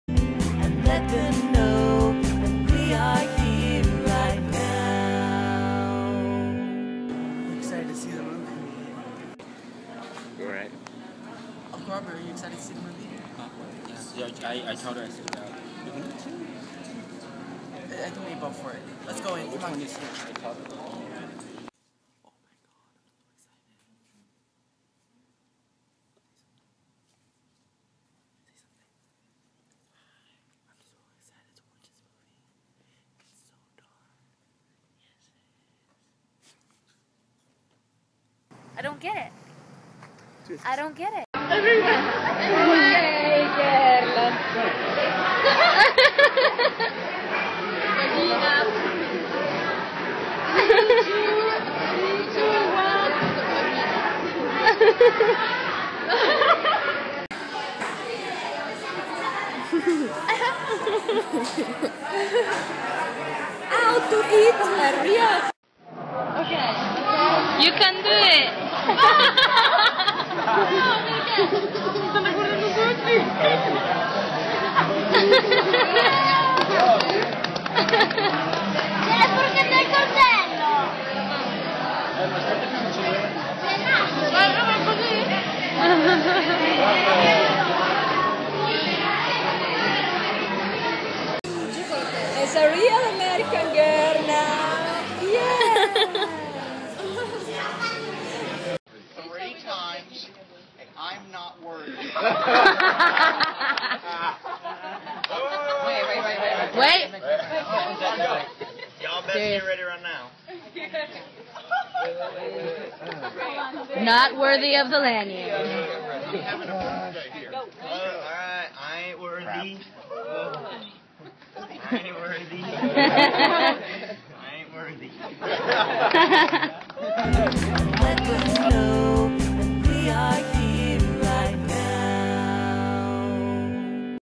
Db Production's "Mini Movies", Season 1, Ep 3: Movies,Burgers, & Pushups
Catch up with Dom in Washington, and Joey in Europe. This episode of mini movies, leaves you wondering, what were they up to this summer?